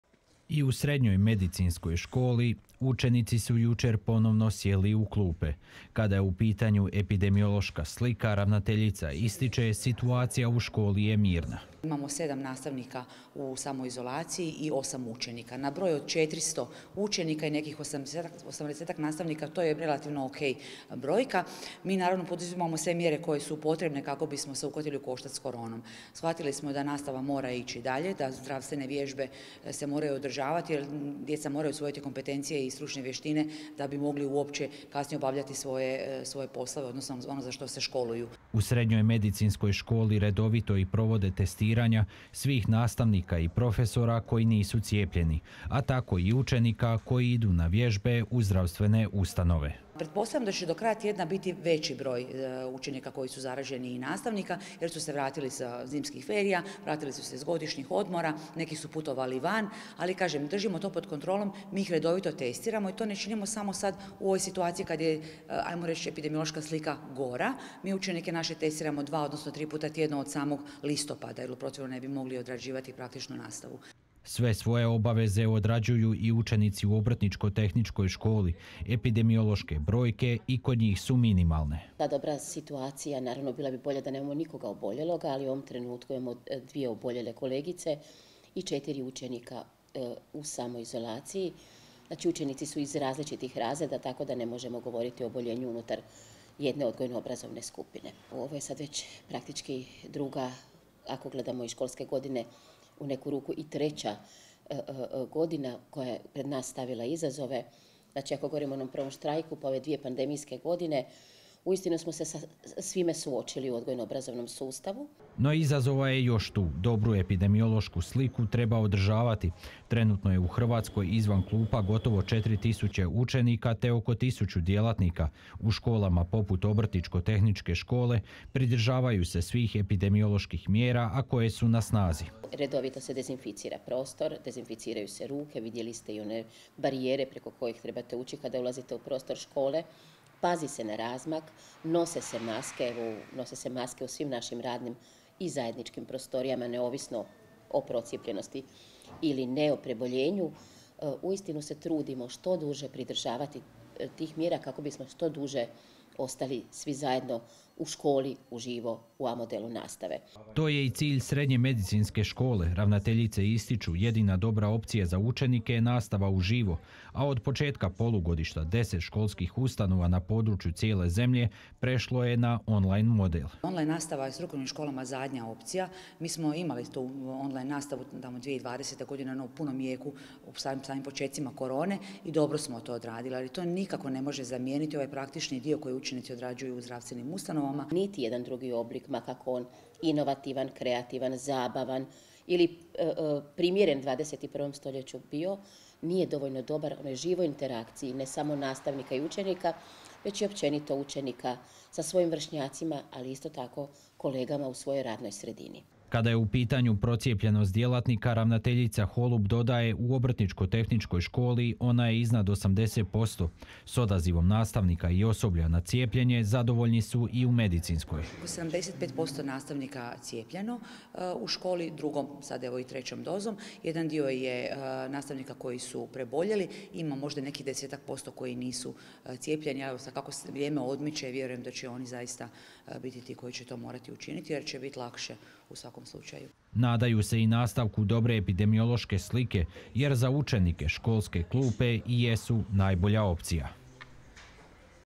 SBTV - DNEVNIK - EPIDEMIOLOŠKA SITUACIJA U NAŠIM SREDNJIM ŠKOLAMA TRENUTNO STABILNA - 11.01.2022.
Jučer je startalo drugo školsko polugodište. No, peti val epidemije stvara poteškoće i u školstvu. Trenutno je u Hrvatskoj pozitivno na koronavirus gotovo 4000 učenika te oko tisuću djelatnika. Desetak škola prešlo je i na model C, to jest, online nastavu. Kakva je situacija u srednjim školama na području Brodsko-posavske županije, provjerila je naša ekipa.

DNEVNIK SBTV-a | PON-PET | 20:00 i 22:00